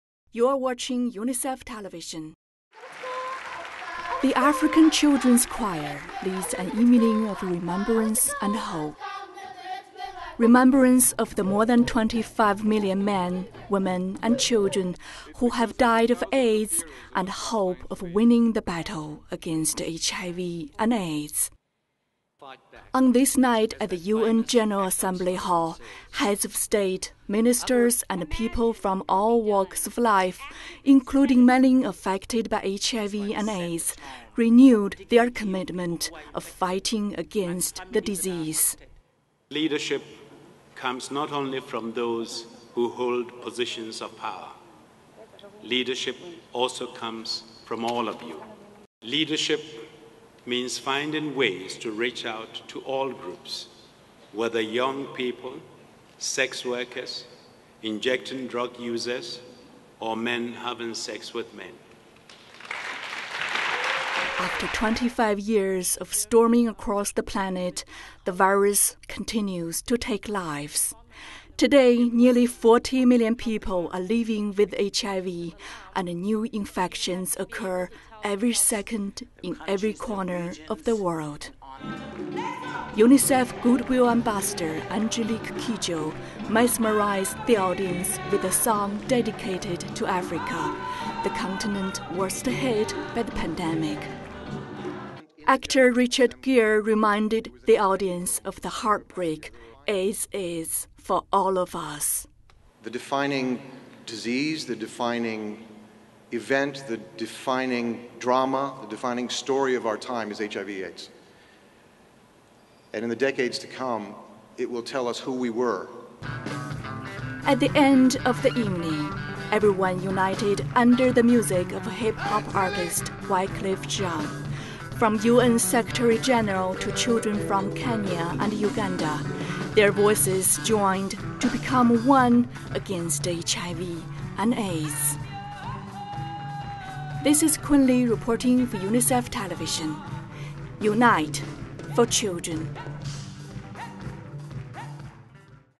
UN partners cap high-level AIDS meeting with night of...
UNICEF correspondent Kun Li reports on an evening of remembrance and hope, held at the UN General Assembly Hall, to mark 25 years of AIDS.  Credits: Producer:kun Li